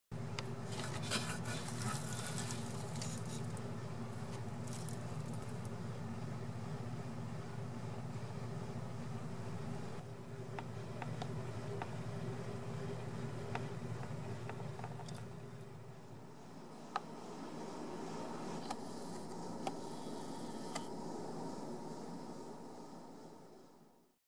Shake it Like a Rattlesnake-Highway Striped California Kingsnake Shows Some Attitude!
Highway Striped California kingsnake "Miles" gives up some rattlesnake tail action!